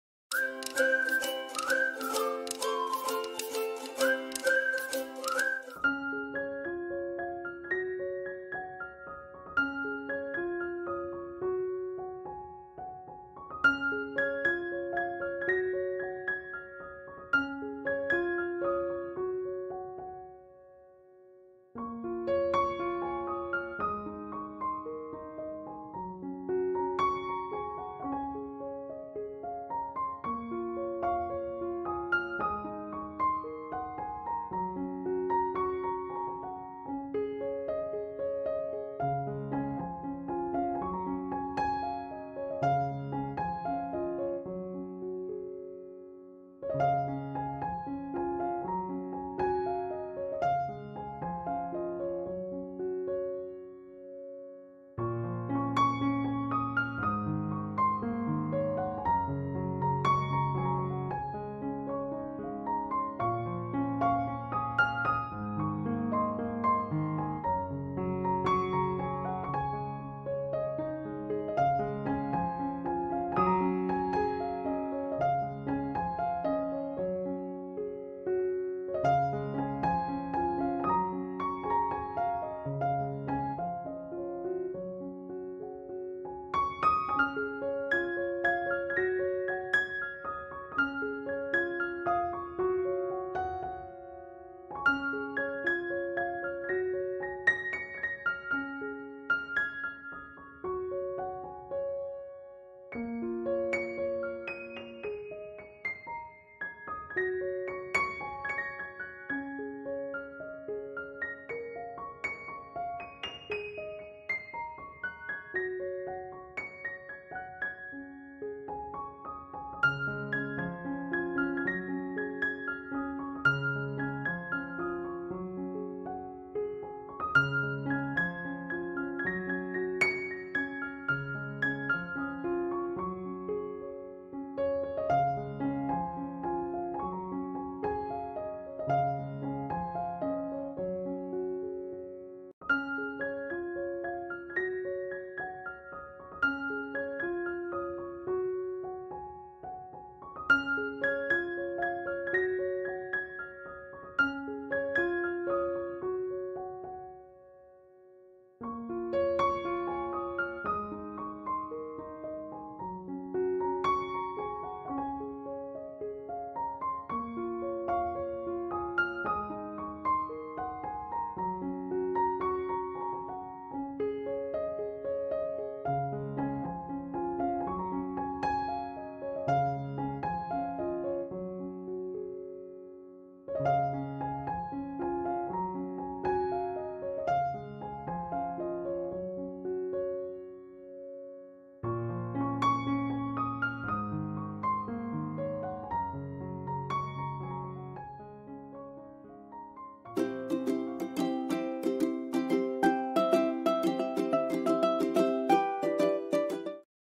Adorable Piglet Digital Watercolor Painting | Autodesk Sketchbook
Facts about Piglet: He loves beautiful things like flowers, is very kindhearted, and is obsessed with keeping things neat and tidy. He sometimes has an inferiority complex, although his friends think highly of him. However, he is often left performing tasks better suited to someone bigger and stronger.